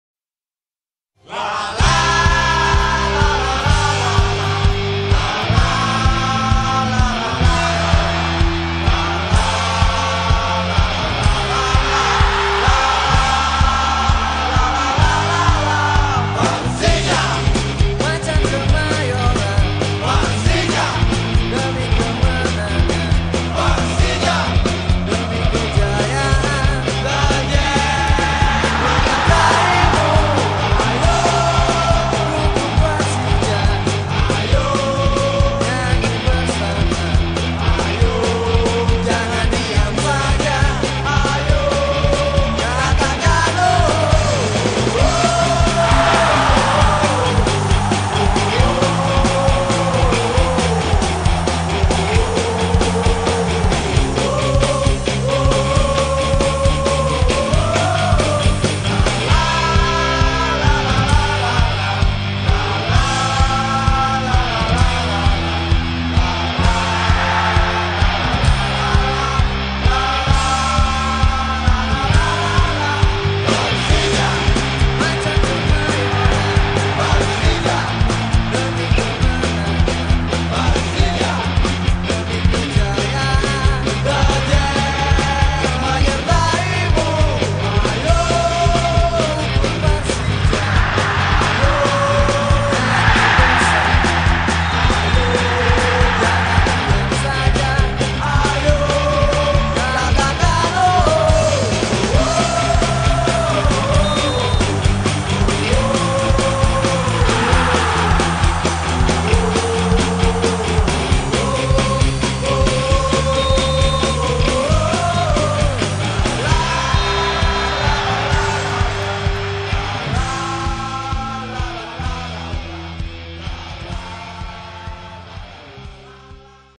Highlight: PERSIJA vs JOHOR DARUL TAKZIM (4-0) • Piala AFC 2018
Cuplikan gol-gol dari Persija saat melawan Johor Darul Takzim, Persija menang telak 4-0.
Marko Simic menjadi Man Of the Match pada laga ini dengan torehan 4 gol.